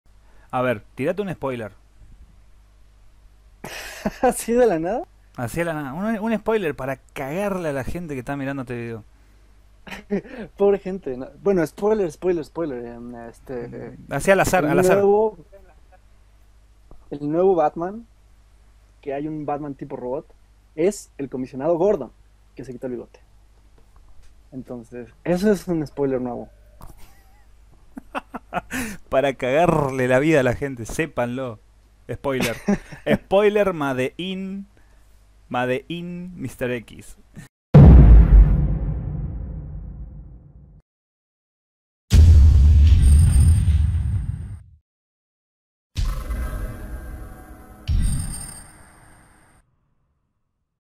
Adelanto de la entrevista a Mr. X de The Top Comic
Mr. X de "The Top Comic" saldrá en modo entrevista para este canal. No te lo podés perder.